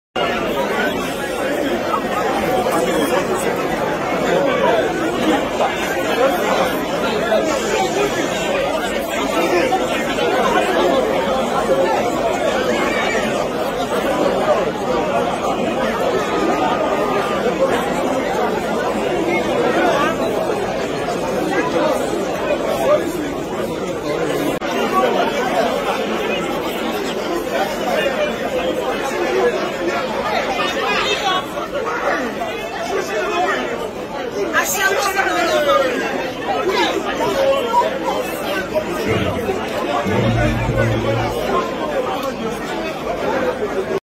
Ex-Gov Obiano's wife slapped by Bianca Ojukwu at the swearing-in ceremony of Prof. Chukwuma Soludo
The Ex-Governor of Anambra State Chief Willi Obiano's wife in an altercation with the wife of Dim Ojukwu, Bianca, at the swearing-in ceremony of the new governor of Anambra State. Prof Charles Chukwuma Soludo.